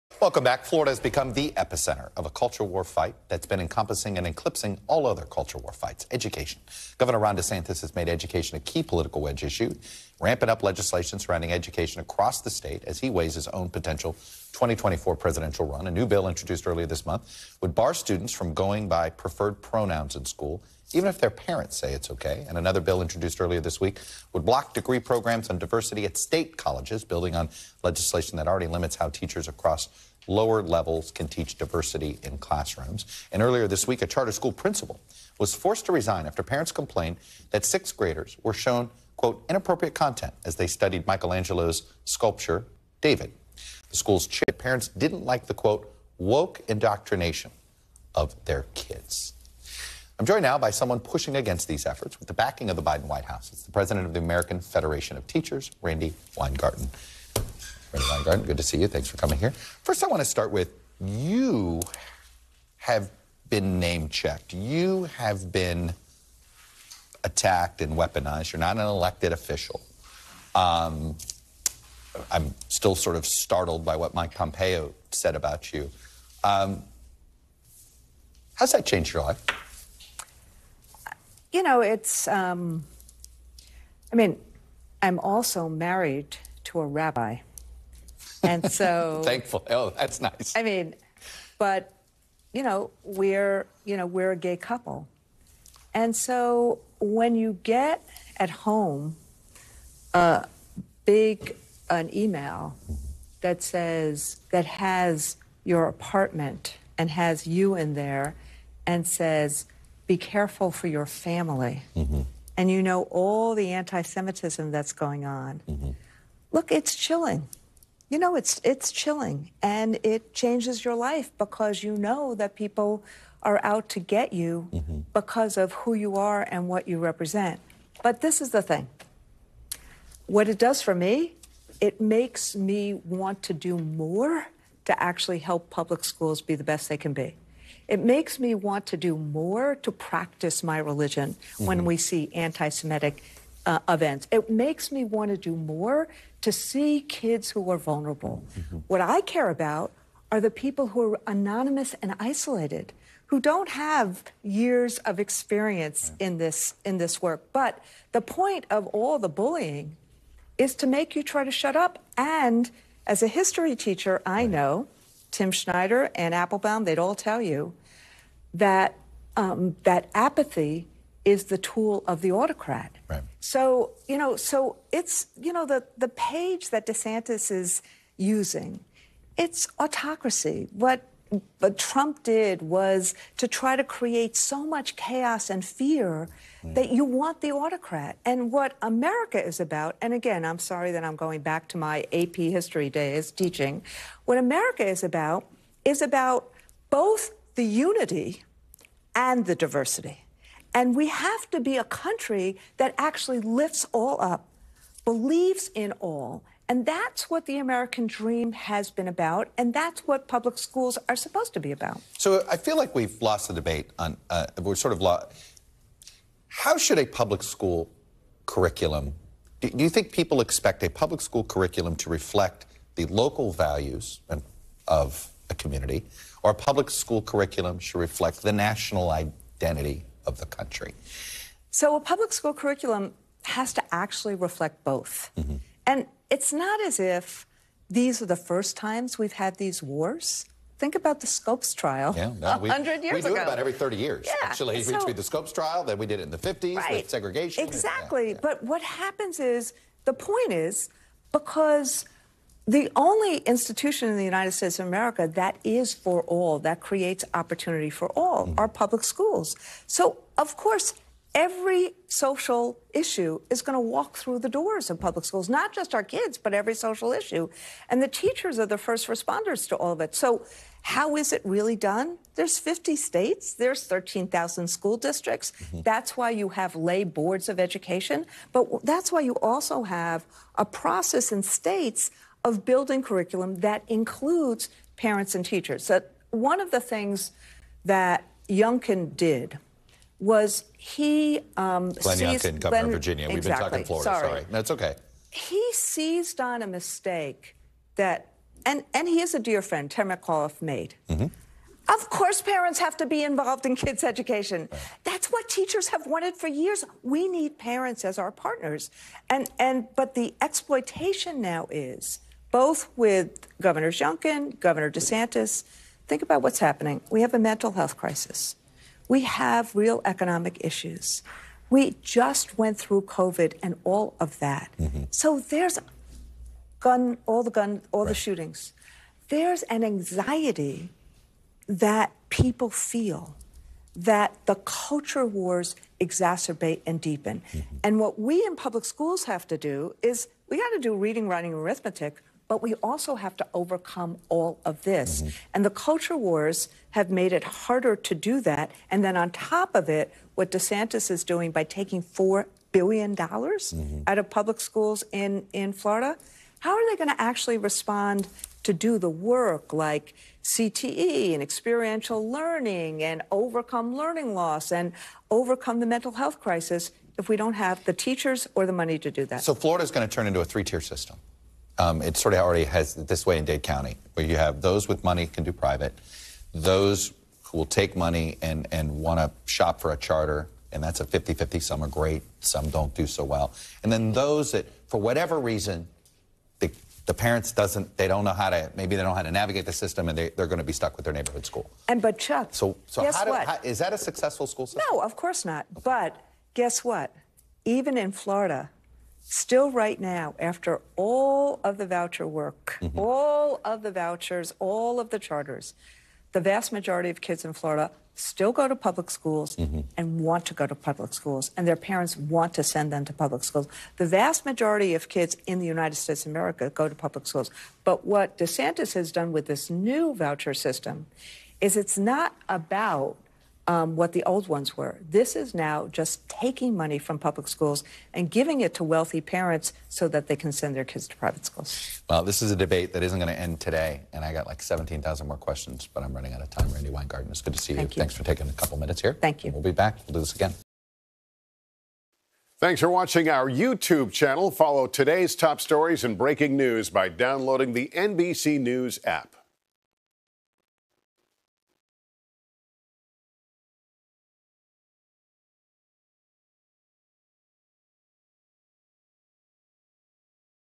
Weingarten: Threats to my family are ‘chilling’ as DeSantis wages war on 'woke'
As Ron DeSantis (R-Fla.) pushes against “woke indoctrination,” threats on education advocates are common. Randi Weingarten, president for the American Federation of Teachers, discusses Gov. DeSantis’ education restrictions in Florida public schools and their effects.